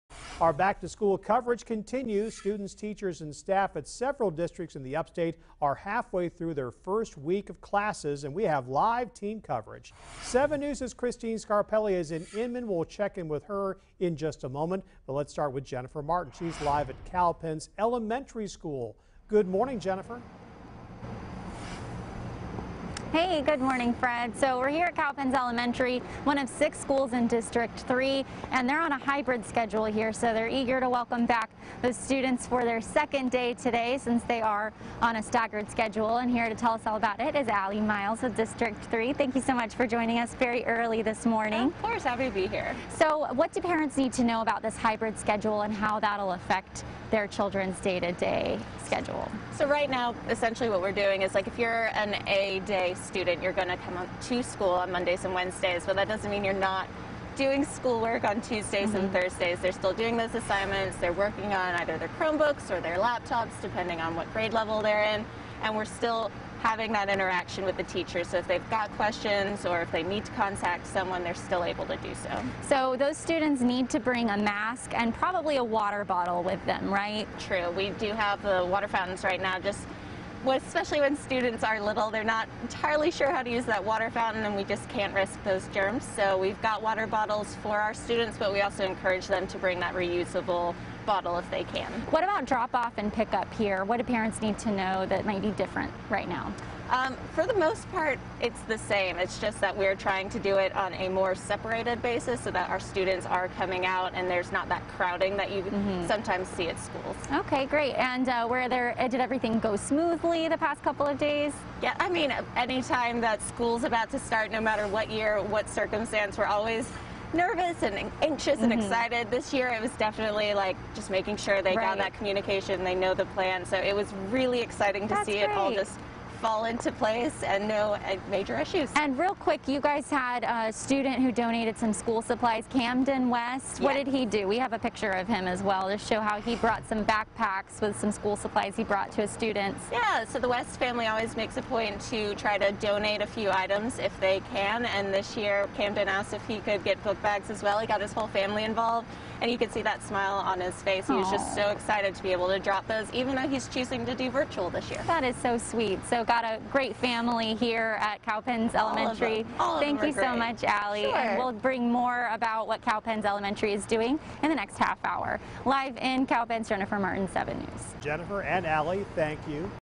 Cowpens Elementary School students adjusting well to new protocol, principal says
Cowpens Elementary School is one of 6 schools in Spartanbug County School District 3, which is operating on a hybrid schedule where one group of students attends in-person Mondays and Wednesdays and another group attends Tuesdays and Thursdays.